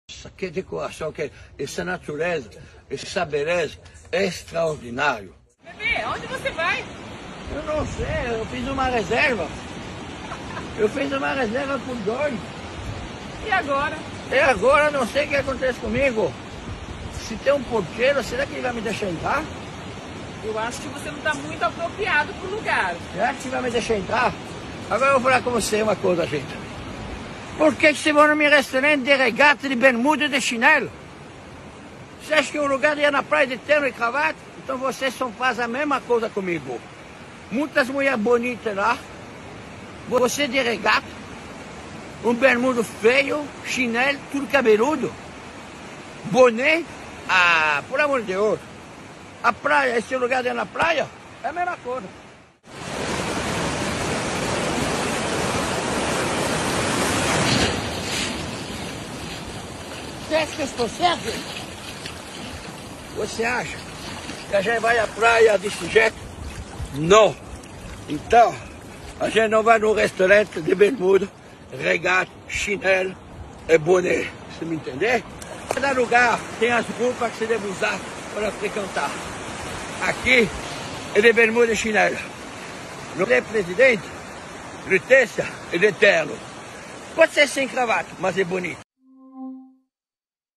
Jacquin vai à praia de terno para criticar clientes que usam bermuda e chinelo em seu restaurante
O renomado chef de cozinha Erick Jacquin surpreendeu ao fazer um 'protesto' inusitado na praia, onde compareceu vestido de terno e gravata para criticar clientes que frequentam seus restaurantes de forma informal.